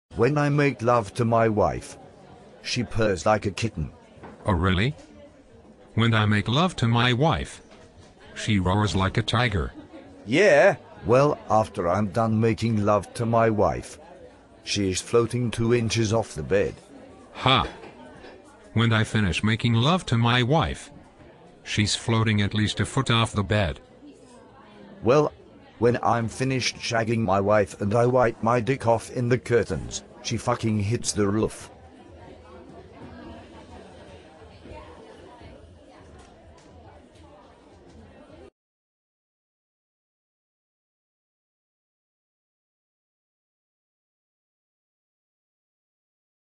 Making Love Competition
Two guys tell stories to see who's better at making love to their wives.